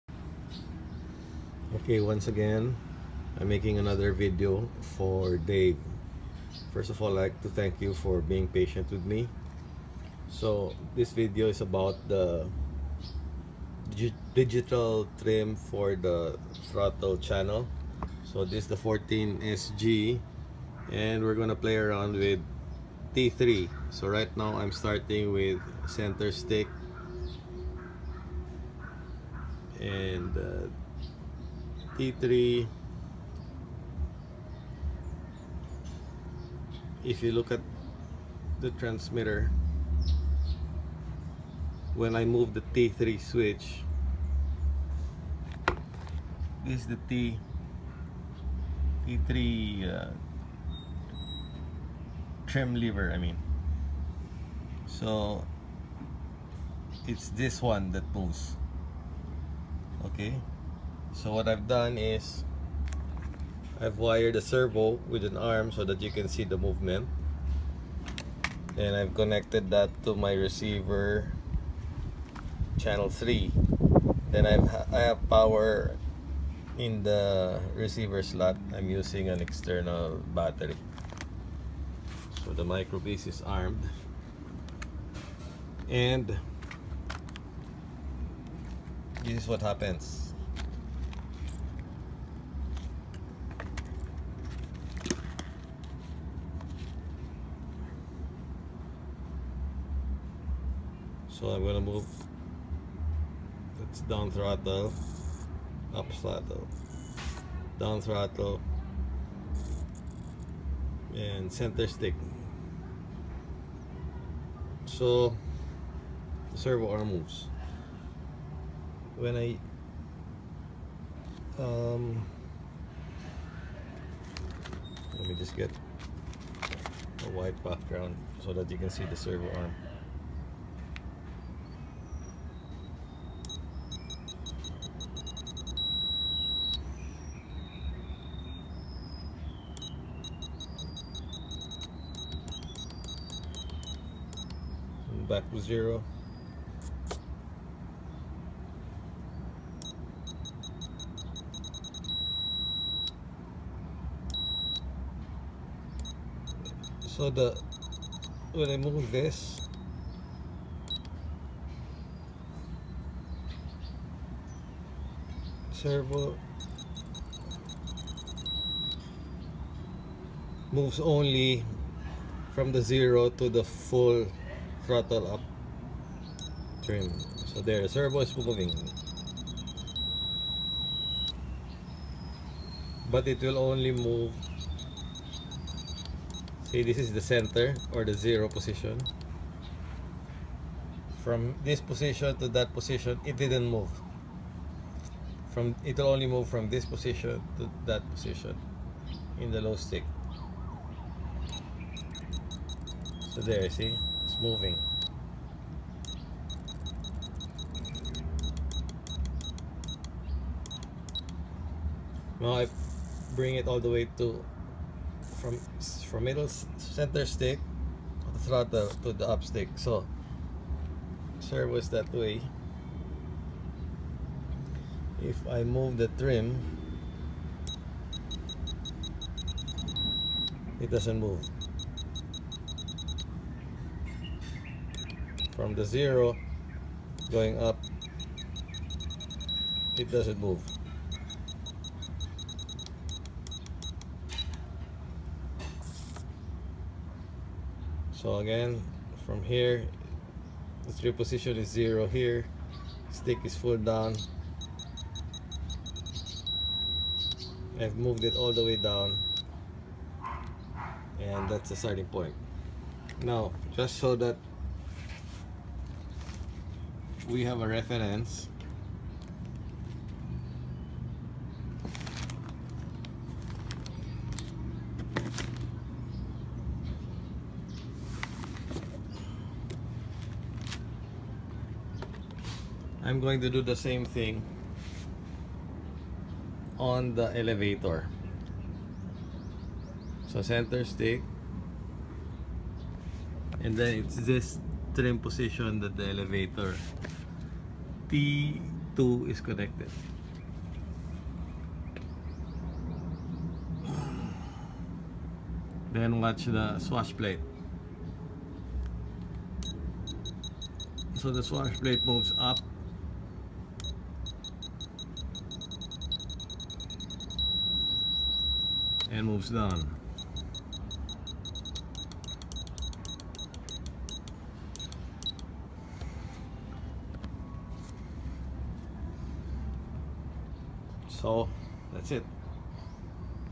trim 782DFBEB 442A 4A36 A606 68278010766D
Setting throttle trim T3 on Futaba 14SG